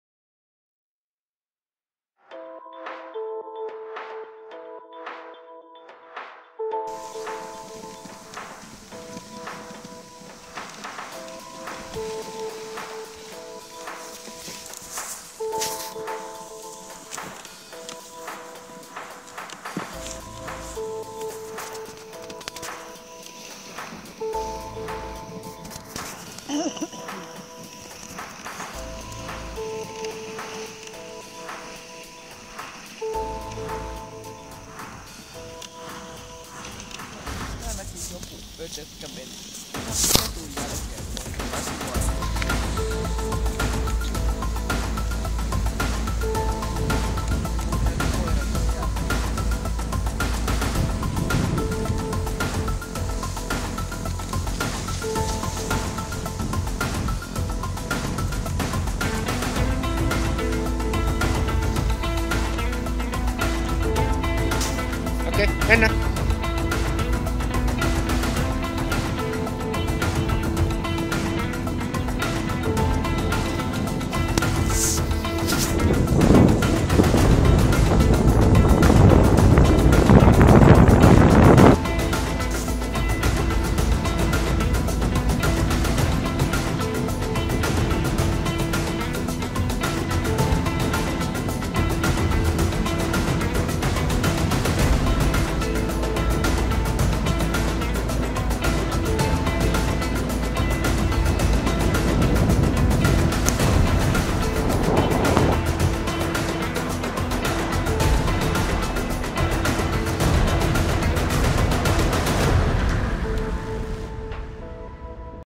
South finland. My mix team 5mals and 3huskies
Lead dogs: Oiva (AM)&Nukka(AM)
Swing dogs: Pate(SH)&Tatsi(SH)
Team dogs: Omayok(AM)&Nippi(AM)
Wheel dogs: Koodi(SH)&Atchuu(AM)